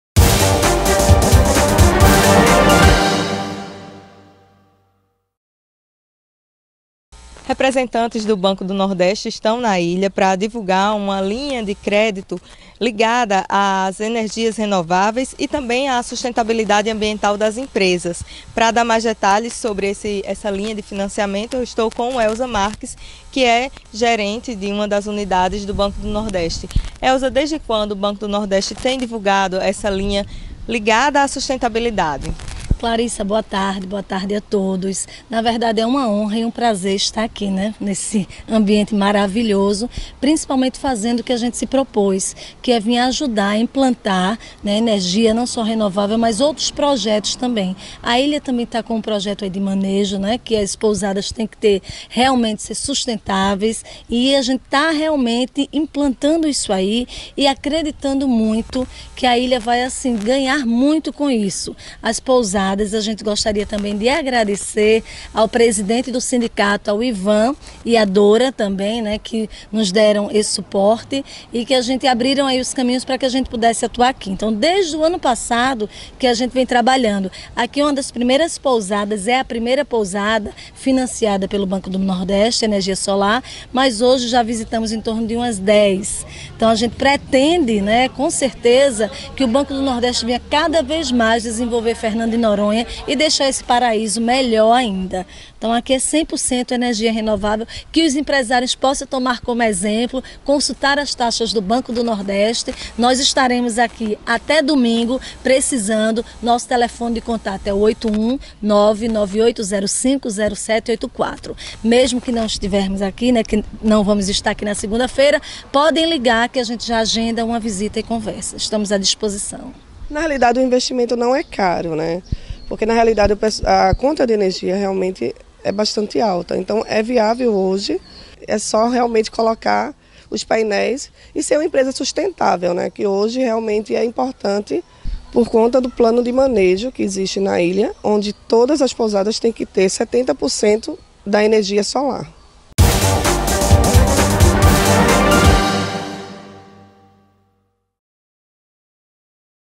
Radar Notícias - BNB anuncia linhas de crédito para projetos de energias renováveis em Noronha
Informativo veiculado dia 12/07/19